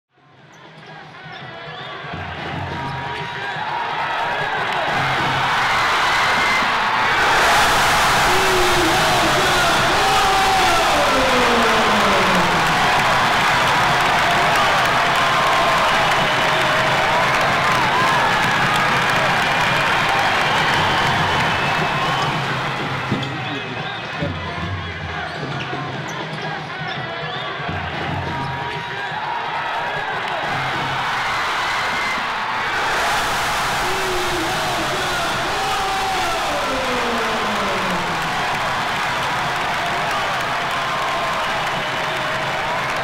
Elijah Clarances DUNK vs. Danmark! 💥
Här är ögonblicket när Baltiska hallen exploderade! 💥
(och du kommer vilja ha ljudet på för den här 🔊🔊🔊)